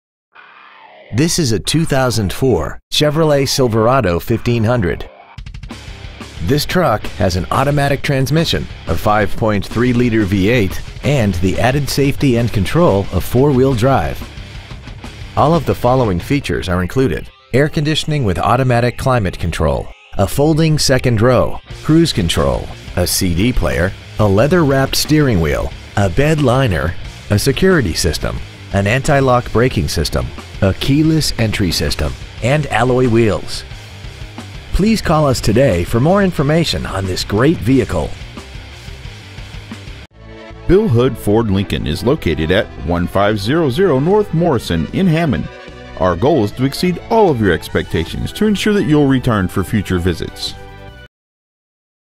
2004 Chevrolet Silverado Covington LA
Year : 2004

 Make : Chevrolet

 Model : Silverado

 Engine : 5.3L V8

 Trans . : Automatic

 Exterior : Green

 Miles : 86,361

 Interior : Gray

 Stock : RP3930A

 1500 N. Morrison

 Pre-Owned 2004 Chevrolet Silverado Hammond LA